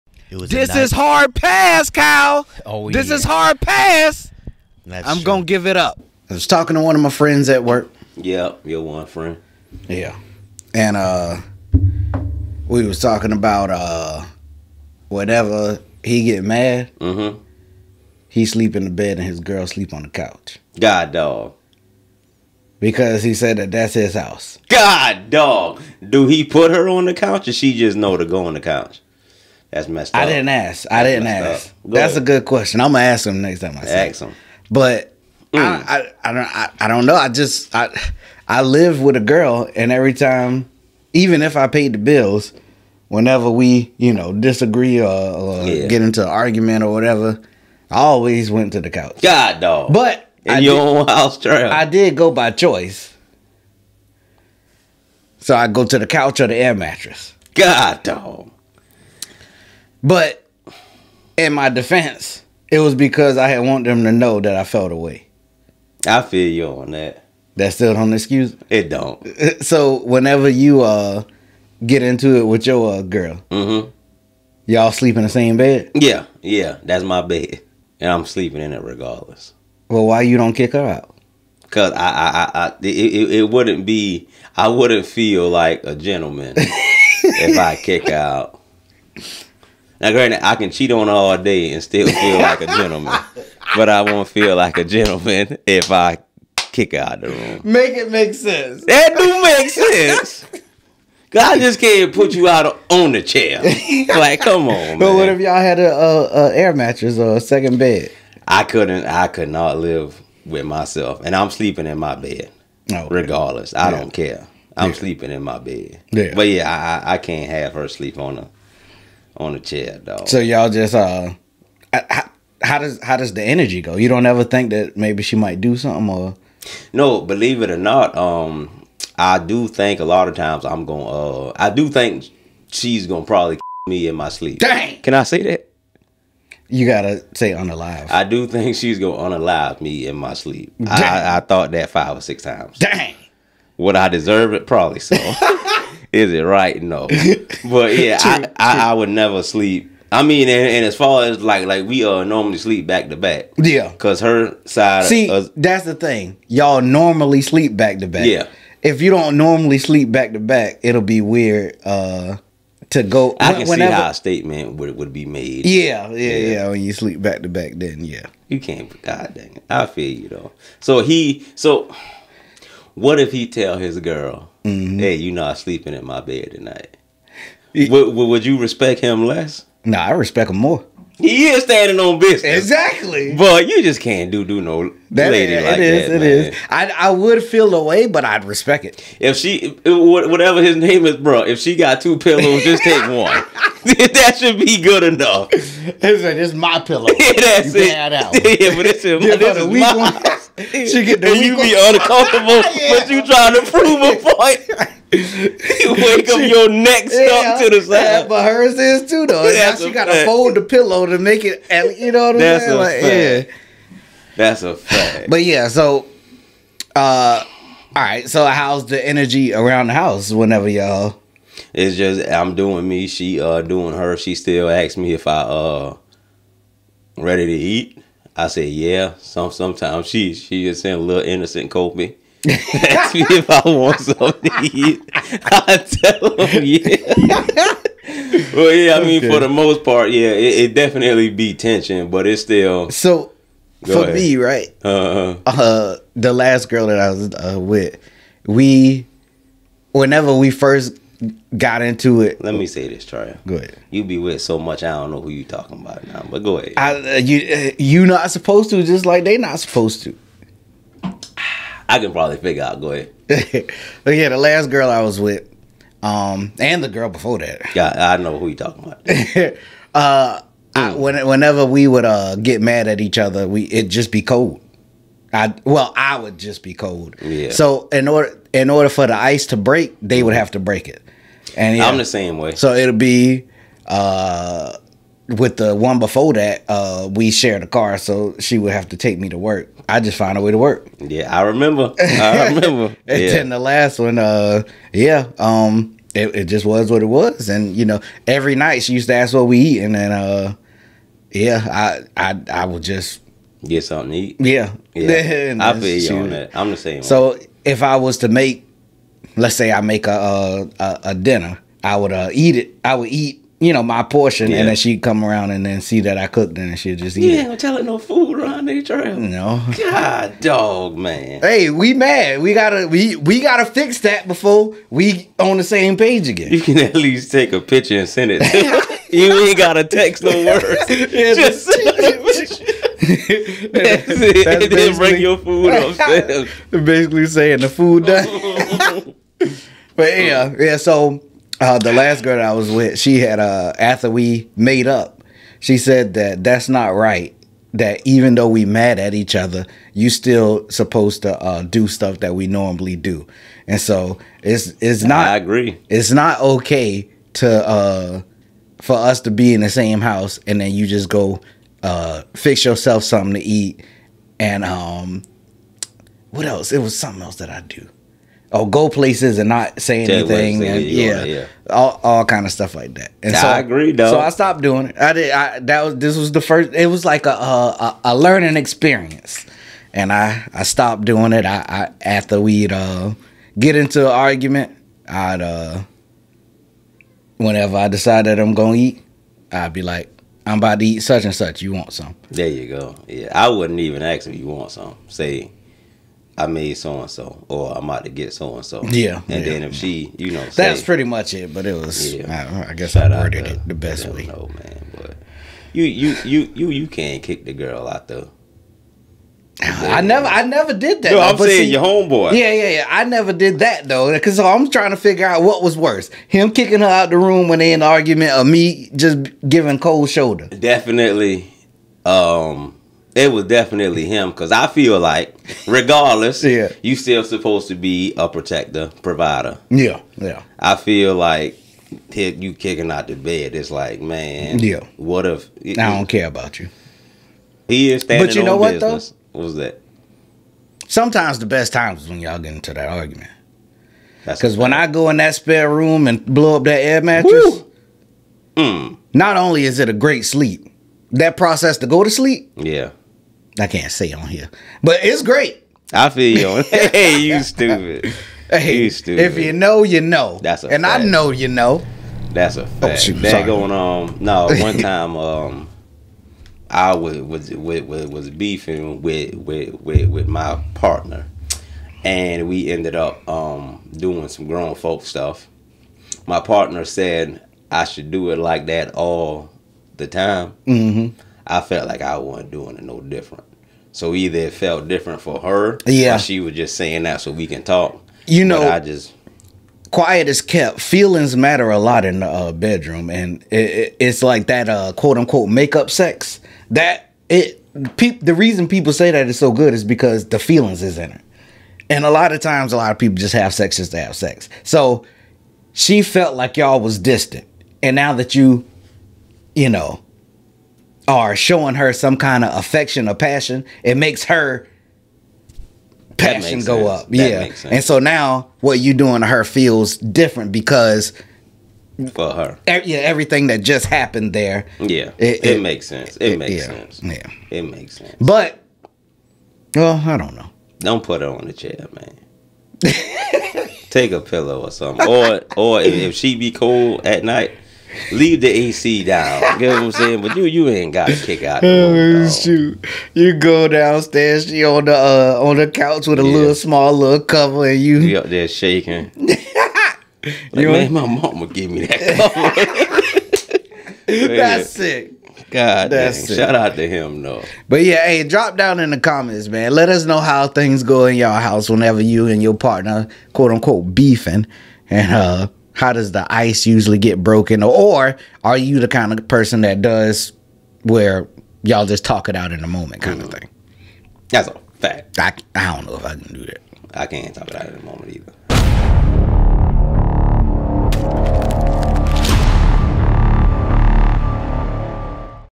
How To Deal With RELATIONSHIP PROBLEMS!!!
ThiWe discuss how we deal with relationship problems in this video joebuddenpodcast newroryandmal needtoknow needtoknowpodcast